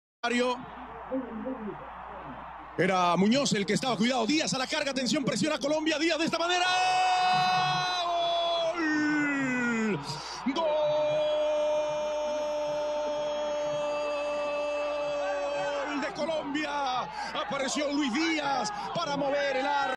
Golazo de Luis Díaz vs Argentina | GOLAZO de Colombia para conseguir empate en SEMIS de Copa América
La definición de Luis Diaz es impresionante. Cayendo mantiene el equilibrio y la pica sin mirar a Martinez. Intuye la salida. Golazo de Colombia.